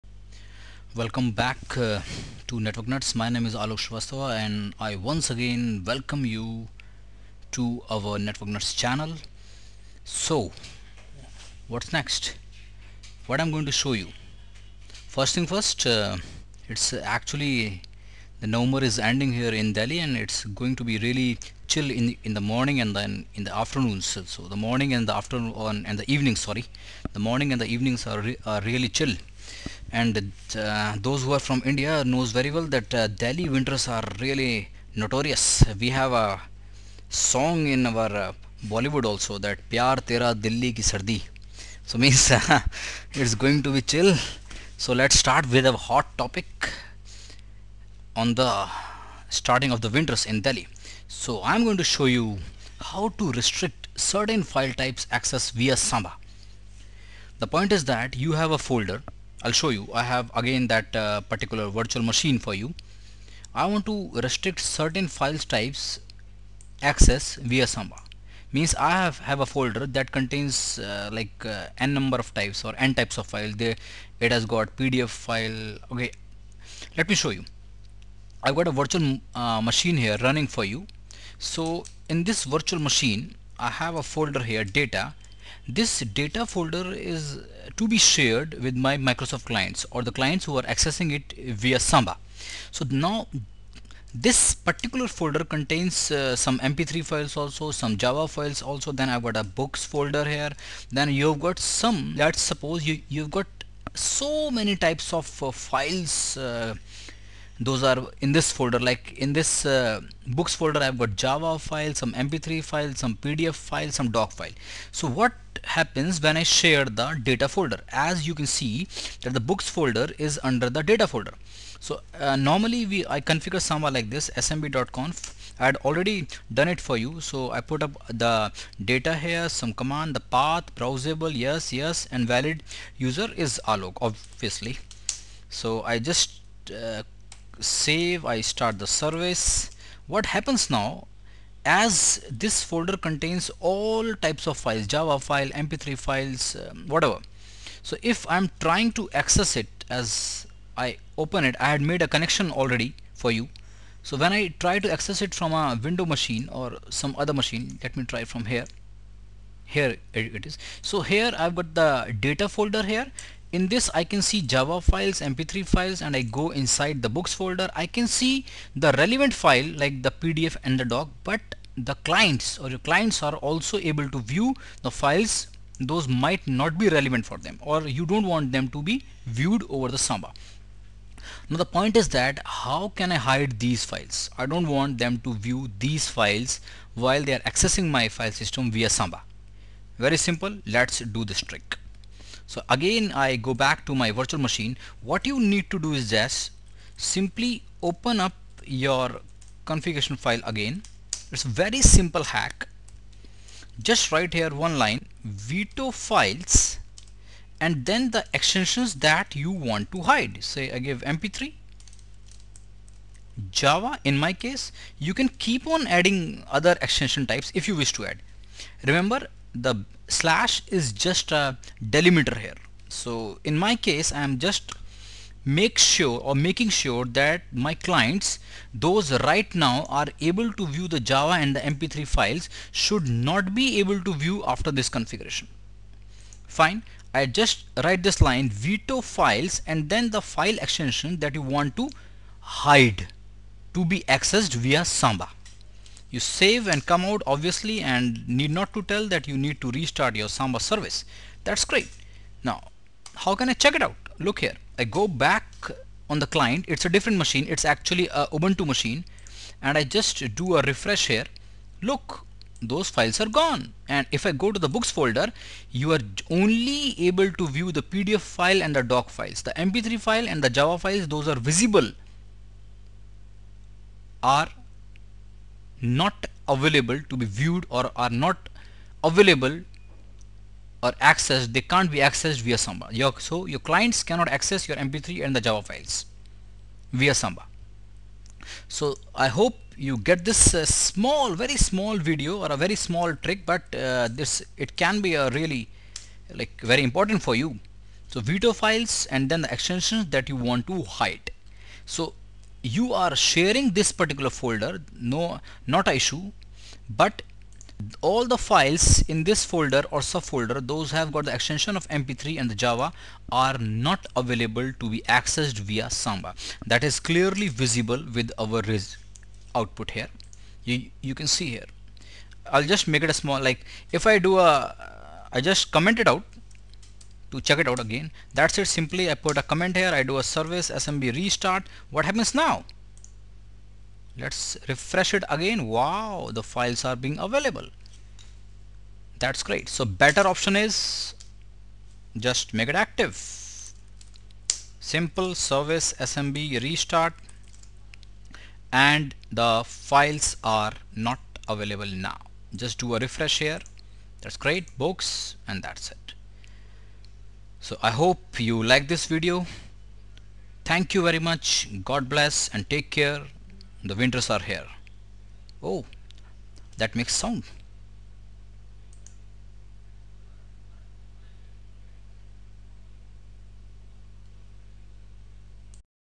Online Tutorials of Hiding certain file types accessing via Samba | Red Hat Linux
video by Network NUTS, shows how to hide certain file types while accessing a share using SAMBA. Thus hiding certain file types you don't want clients to view.